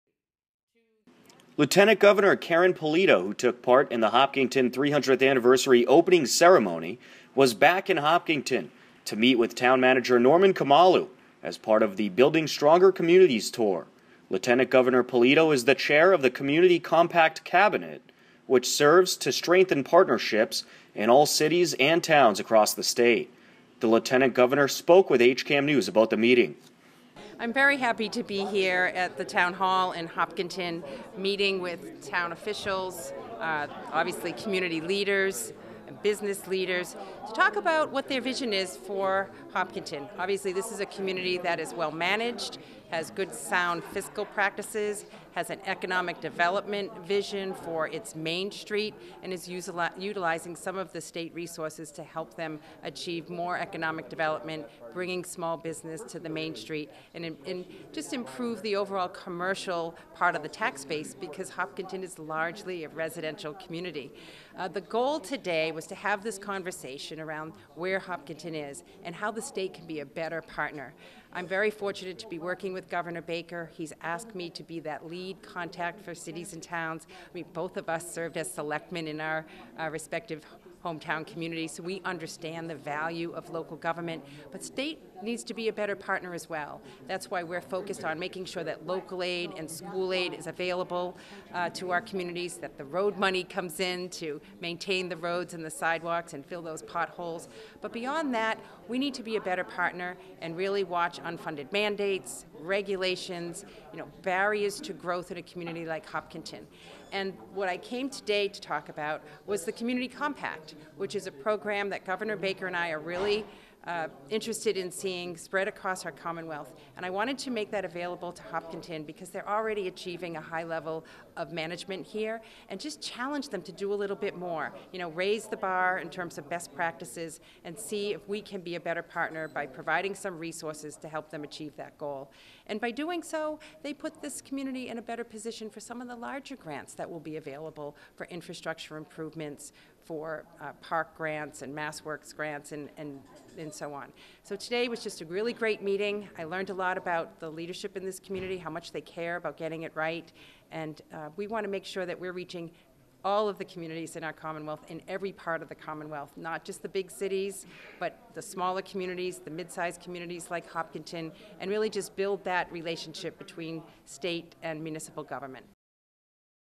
Lieutenant Governor Karyn Polito Visits Hopkinton
Lieutenant Governor Karyn Polito who took part in the Hopkinton 300th anniversary opening ceremony was back in Hopkinton to meet with town Manager Norman Khumalo as part of the  "Building Stronger Communities" tour.

Lieutenant Governor Polito is the Chair of "The Community Compact Cabinet" which serves to strengthen partnerships in all cities and towns across the state.

The Lieutenatnt Governor spoke with HCAM News about the meeting.